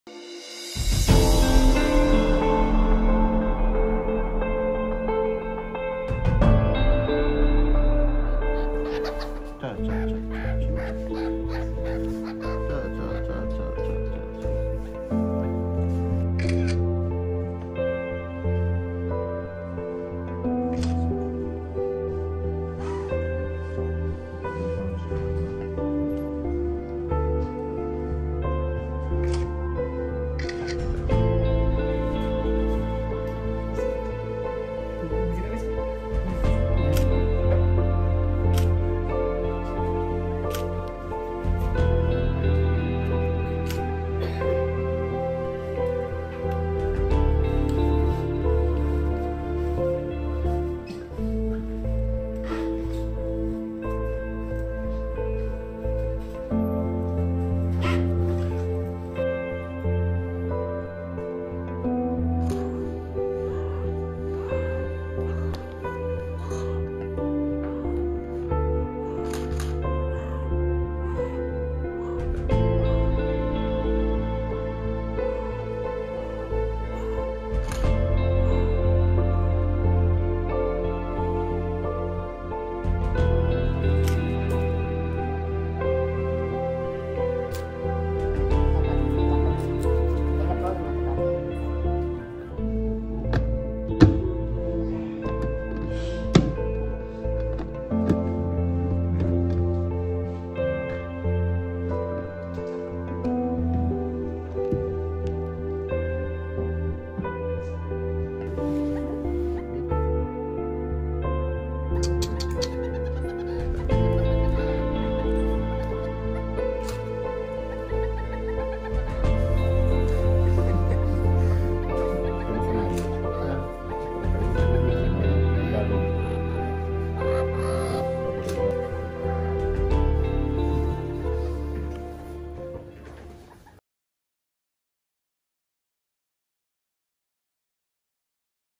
Rare wildlife species seized at Chennai airport
Exotic wildlife including meerkat, fat tail gerbil, green iguana and even tarantula spiders which were allegedly being smuggled in from Bangkok, were seized at the Chennai airport.  The animals and insects were found inside plastic boxes inside a person's luggage bag.